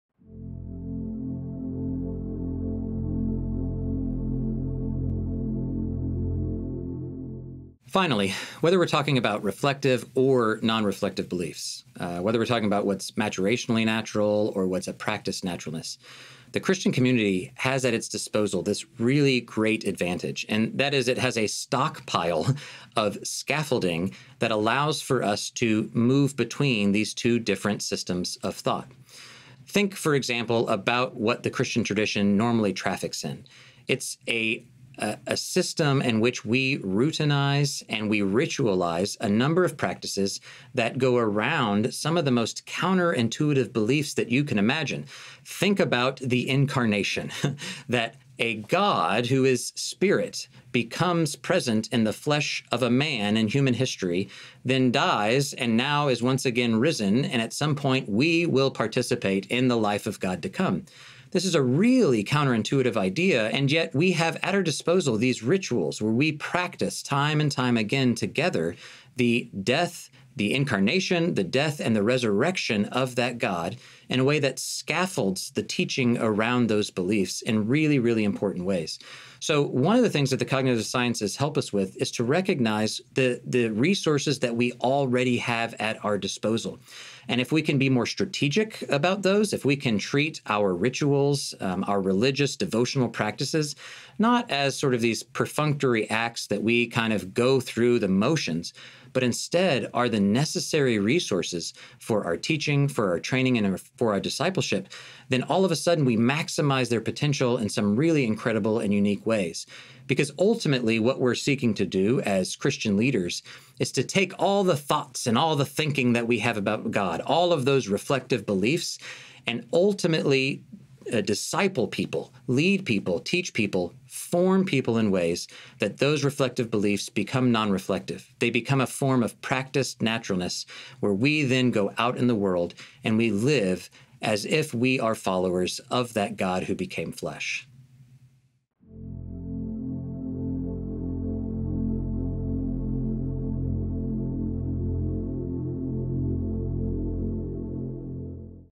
The Cognitive Sciences and Belief: Christian Ritual As Scaffolding
In the last of nine videos on “The Cognitive Sciences and Belief,” Kutter Callaway summarizes how understanding the cognitive sciences in relation to our faith helps ministry leaders in areas of teaching and discipleship.

Kutter Callaway is the William K. Brehm Chair of Worship, Theology and the Arts and associate professor of theology and culture.